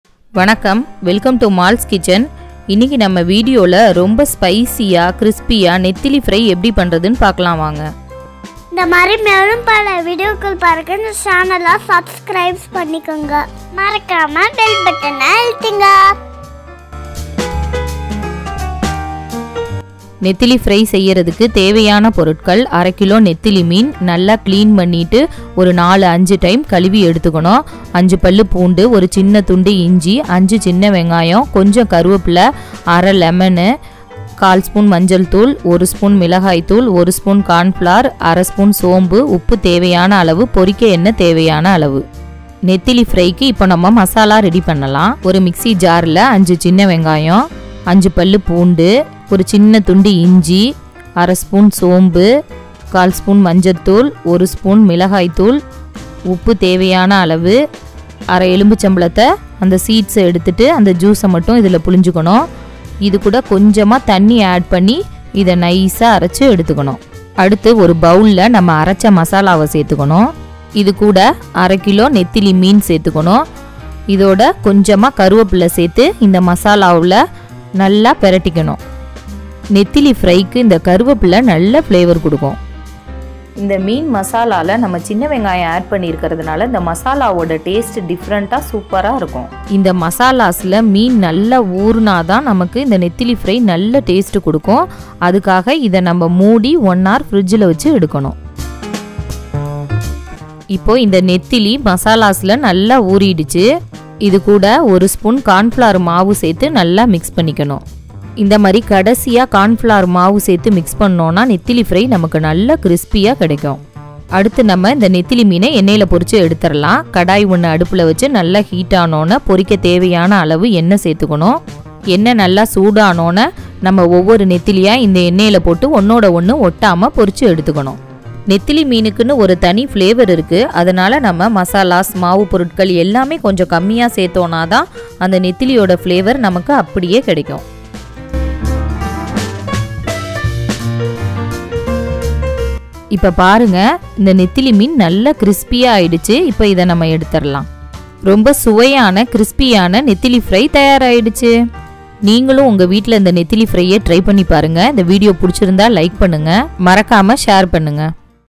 மொறு மொறுப்பான நெத்திலி மீன் ஃப்ரை| Crunchy & Crispy Nethili Fish Fry
சுவையான நெத்திலி மீன் ஃப்ரை, ஸ்னாக்ஸ் போல அப்படியே சாப்பிடலாம்.
Crunchy and crispy nethili fish fry.